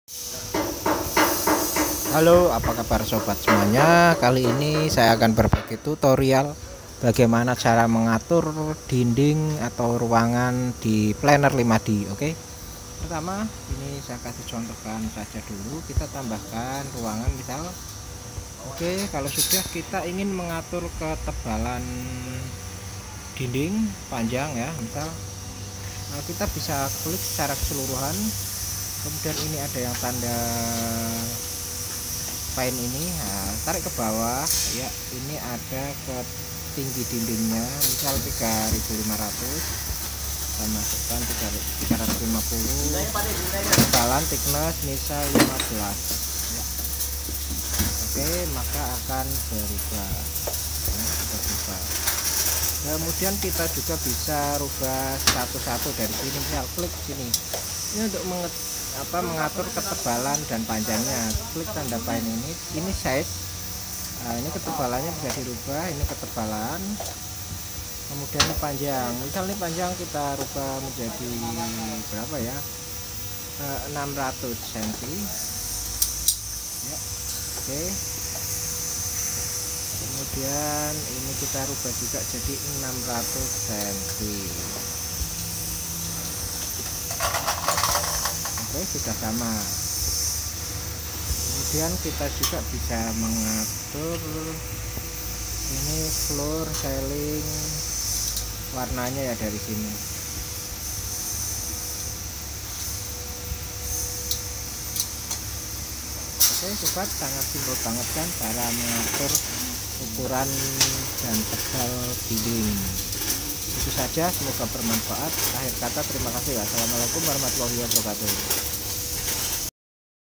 Mengatur Ukuran Tebal dan Tinggi Dinding Planner 5D Aplikasi Desain Rumah 3D Android
Video kali ini adalah tutorial planner5d aplikasi desain rumah 3d yang real banget.